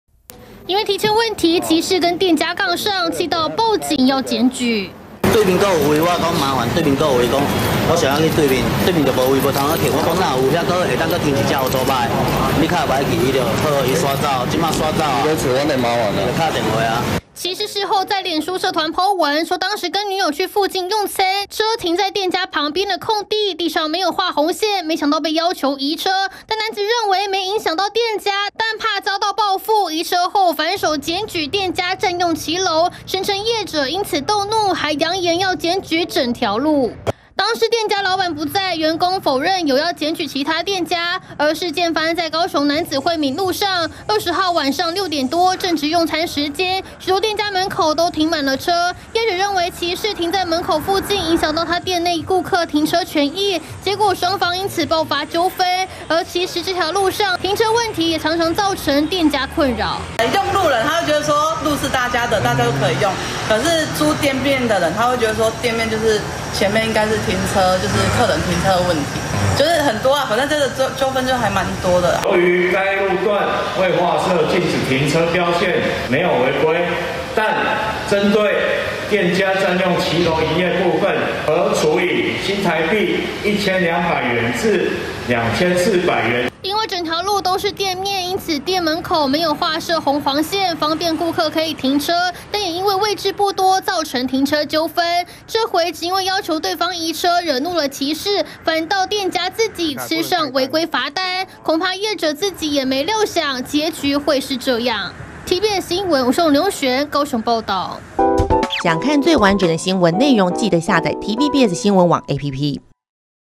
不讓停店門旁! 店家反遭騎士檢舉佔用騎樓挨罰｜TVBS新聞
高雄楠梓有店家因為民眾到附近用餐，非店裡顧客卻把車停在店門口旁，業者請對方停到對面空位，結果引發騎士不滿，移車後報警檢舉店家佔用騎樓，店家因此吃上1200到2400元的罰單。